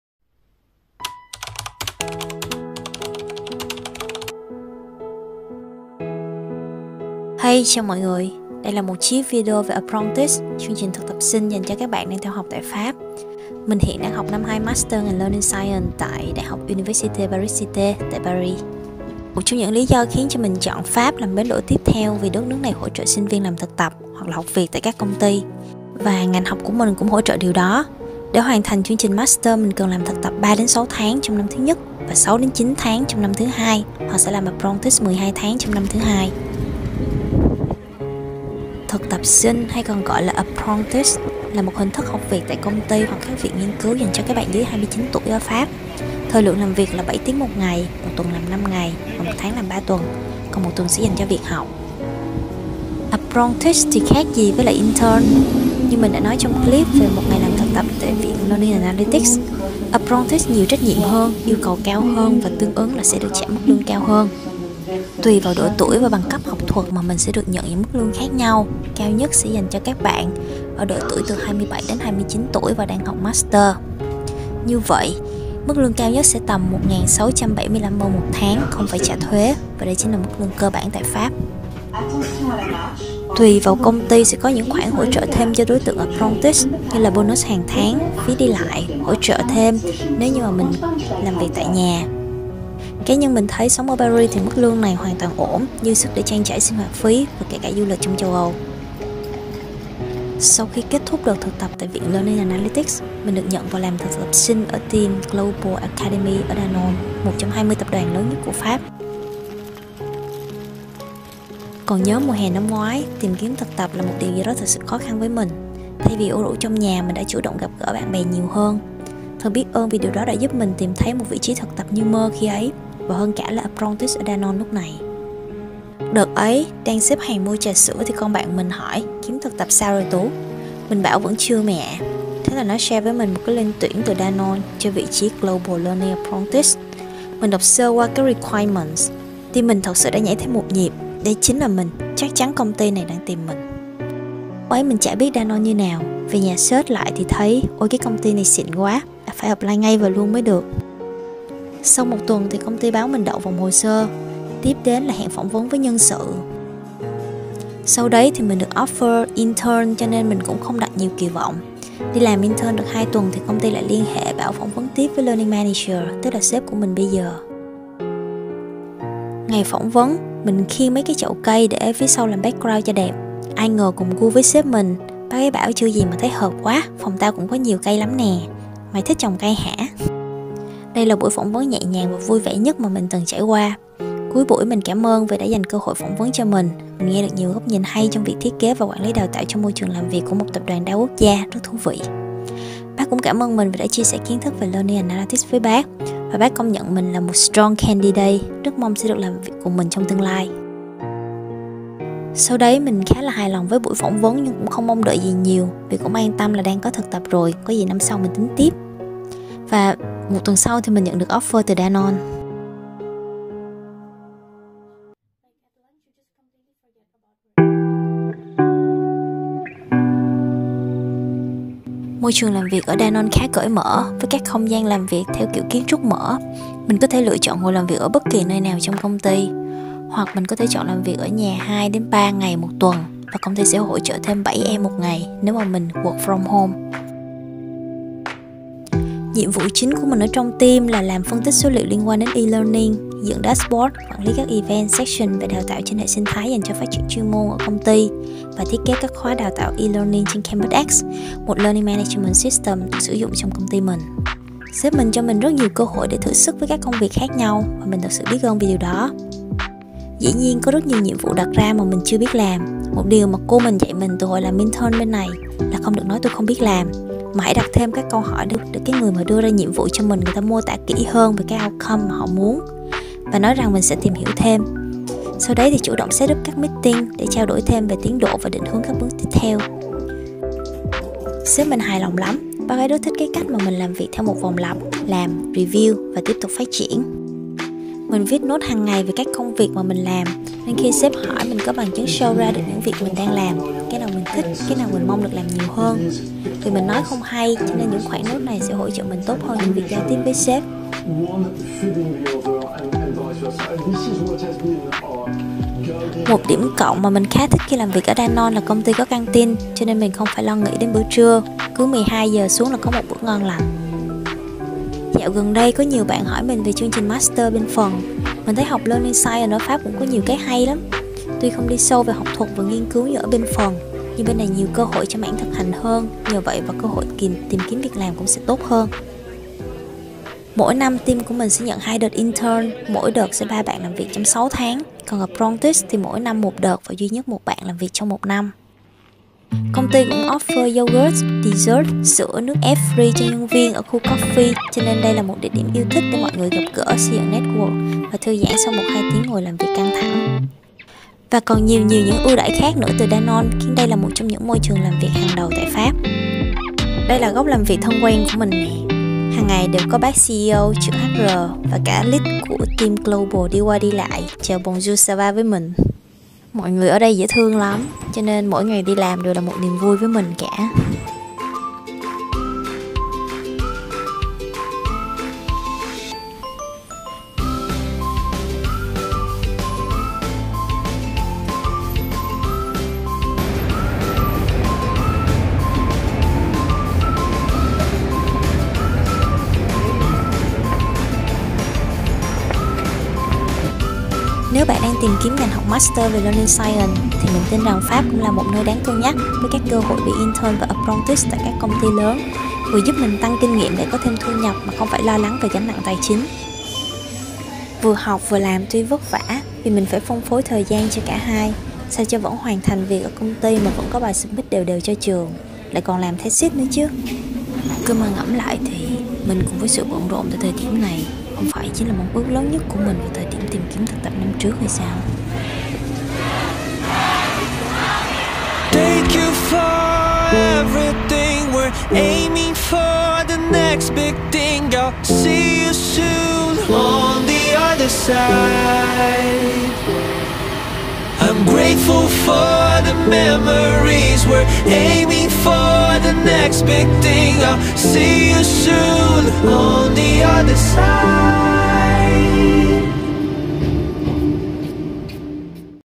Apprenticeship in Danone
Chào mọi người, đây là 1 chiếc video về chương trình Apprenticeship của mình ở Danone - một trong 20 tập đoàn lớn nhất của Pháp. Mong rằng chiếc video có thể gợi ý cho bạn thêm một sự lựa chọn khi tìm kiếm ngành học master về learning science ở Châu Âu.